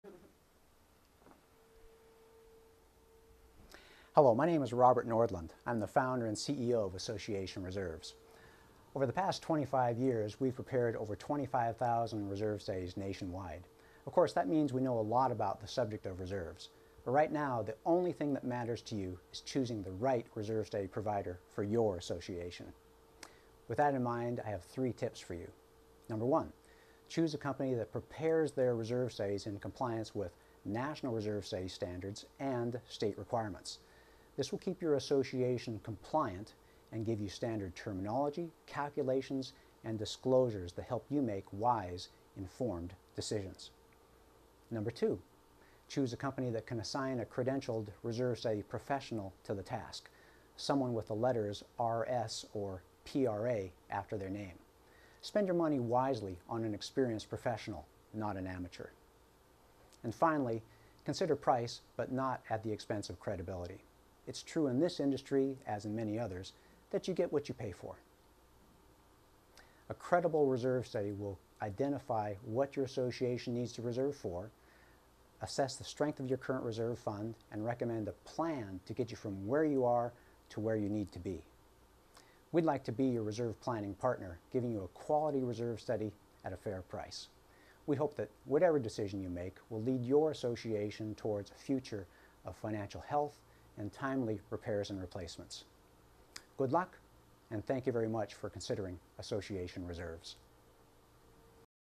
3 Tips for Choosing the Right Reserve Study Provider
Company Founder Robert Nordlund provides three important tips to help in selecting an appropriate Reserve Study provider for your association.